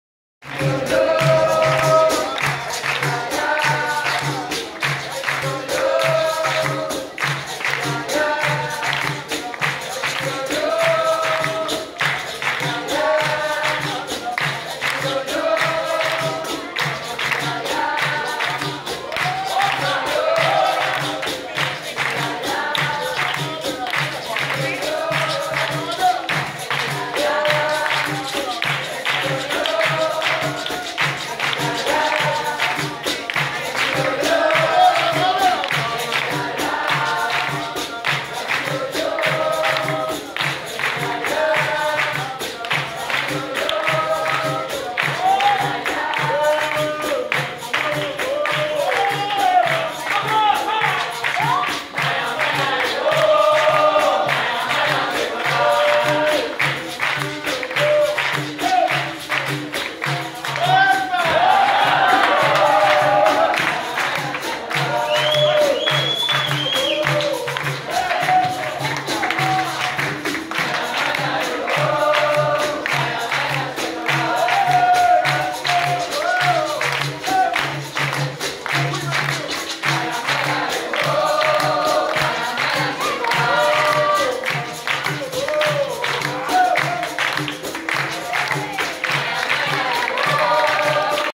Mestre Russo & Mestre Branco - Encontro Capoeira Ginga Nagô Nantes 2016
Roda no Encontro Internacional de Capoeira & cultura brasileira realizado em (St Fiacre) Nantes entre os dias 21 e 24 de Abril de 2016, organizado pelo Mestre Branco e Ginga Nagô Nantes.